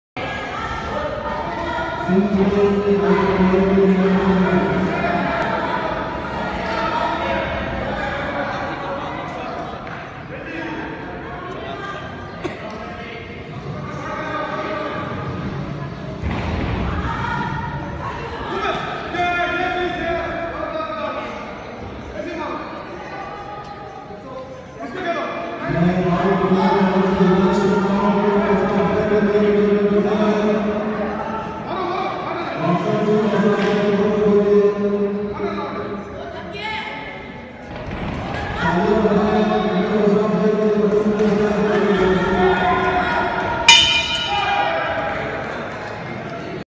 Kashmir Day Boxing Tournament of kids at BBSUL.
Boxing Tournament of kids was organized on Kashmir Day at BBSUL.